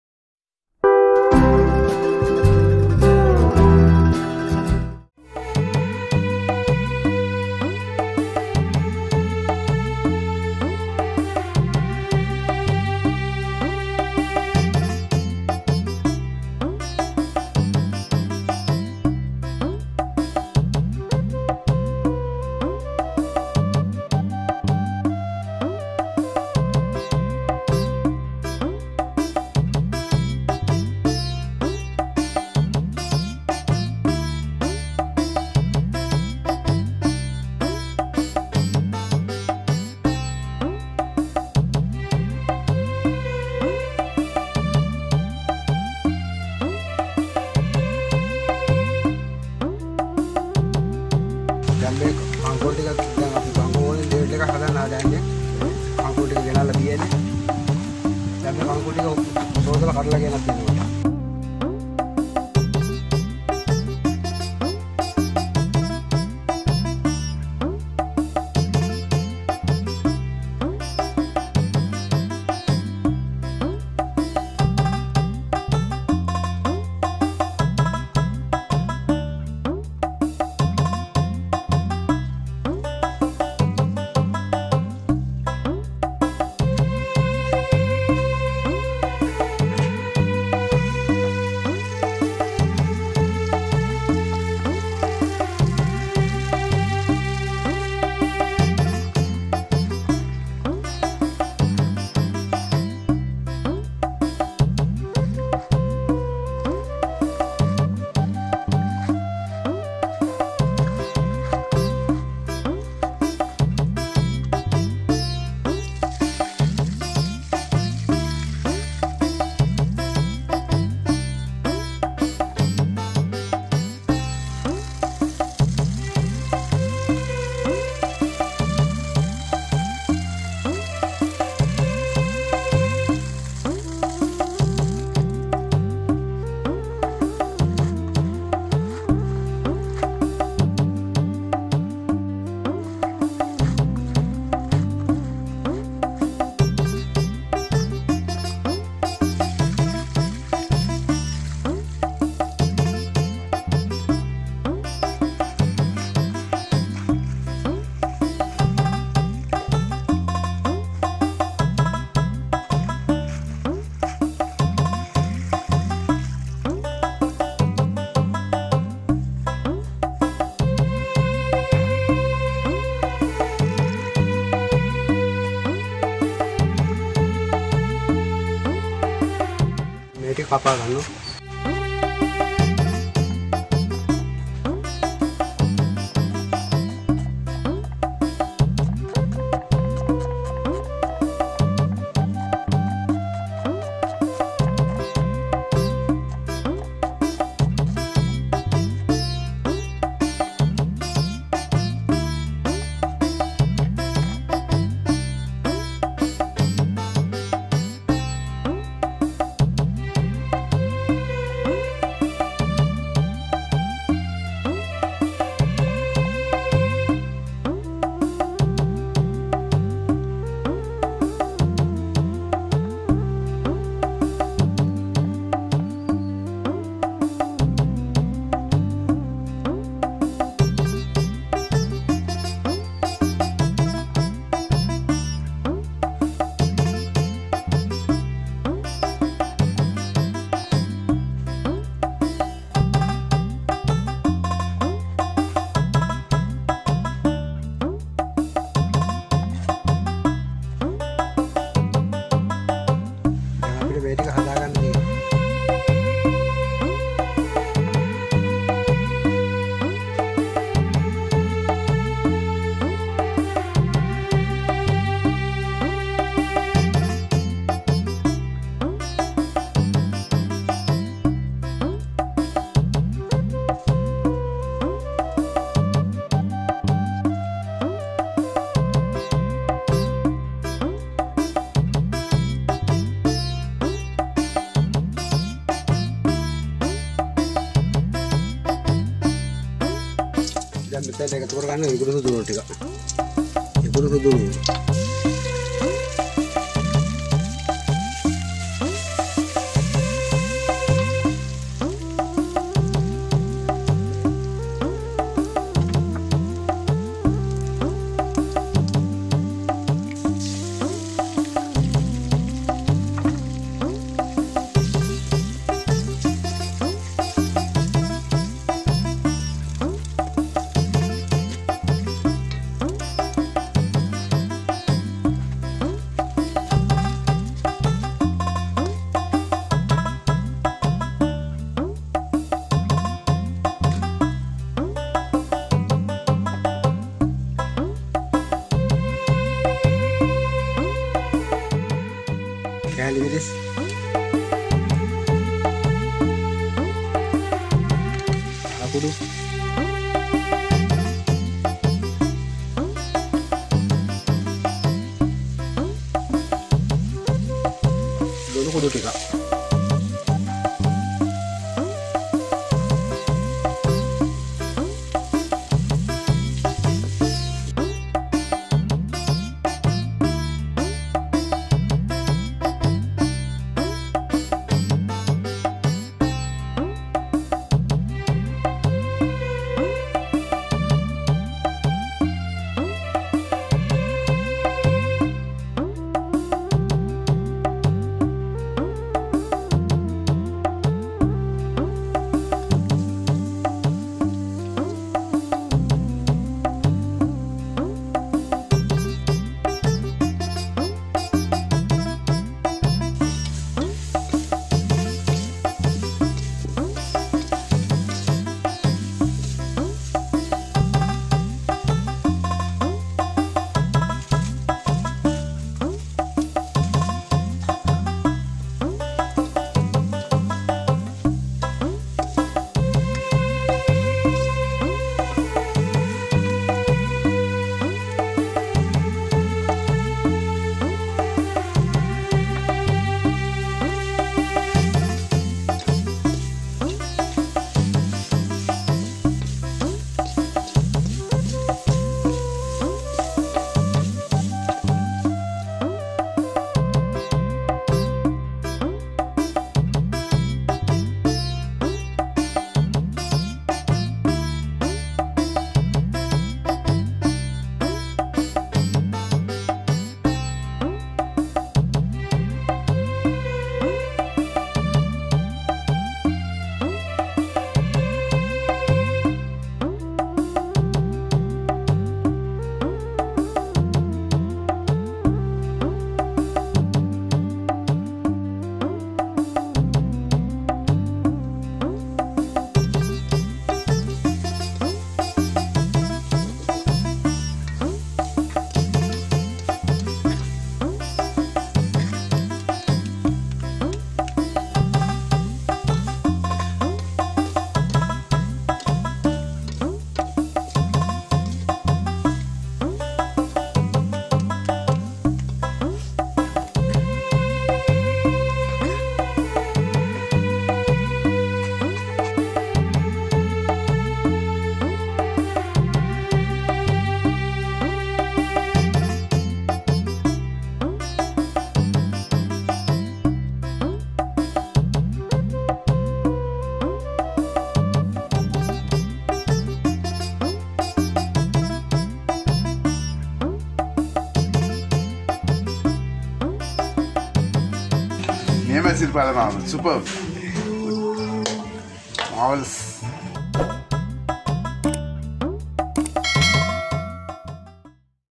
Kankun Devilled - කන්කුන් ඩෙවල්ඩ්
දකුණේ මාතර රහට කන්කුන් ඩෙවල්ඩ් ගෙදරදීම පහසුවෙන් සකස්  කරගන්නා අයුරු මෙම වීඩියෝවෙන් පෙන්වා දෙයි.

This video shows you how to easily prepare Kankun Devilled tastes right in the south.